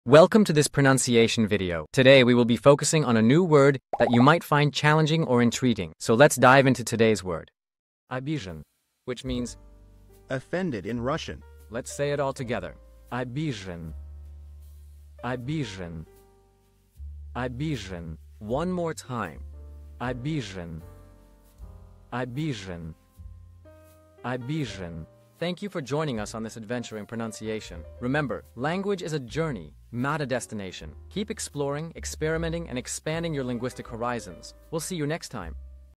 How to pronounce Obizhen | Обижен (Offended in Russian)
Master the Pronunciation of 'Obizhen | Обижен' - which means : Offended in Russian 📝 with @PronunciationRussian 🎓🗣️ - [Your Guide to Russian]

Learn to pronounce 'Obizhen' perfectly in Russian with our easy and engaging tutorial by @PronunciationRussian! 📖📽️ In this comprehensive video, we provide you with native speaker examples 🗣️👂 and phonetic breakdowns 🎵 to ensure you get the pronunciation just right.